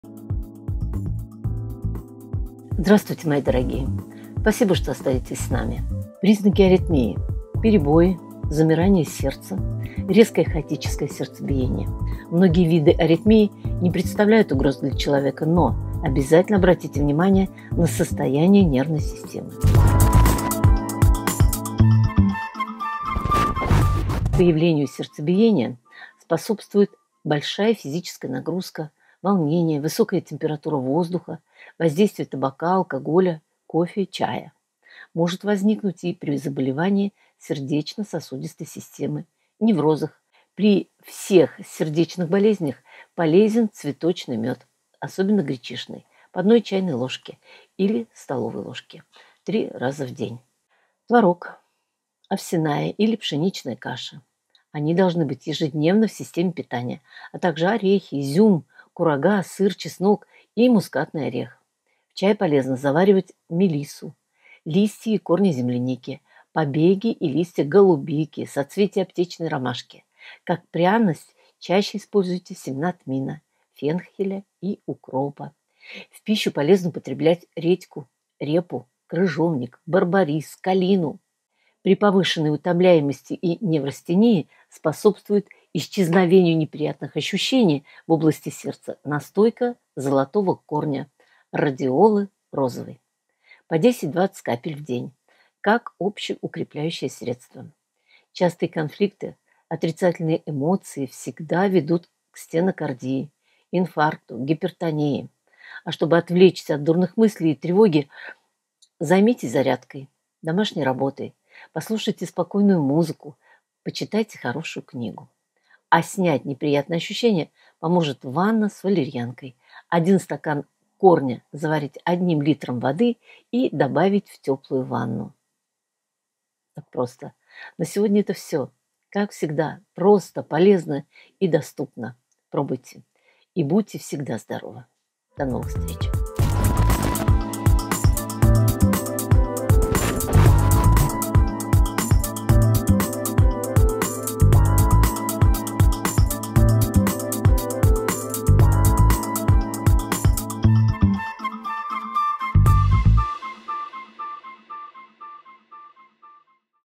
Аритмия болезнь сердца а лечить нужно нервы
Признаки аритмии - перебои, замирание сердца, резкое хаотическое сердцебиение. Многие виды аритмии не представляют угрозы для человека, но обязательно обратите внимание на состояние нервной системы.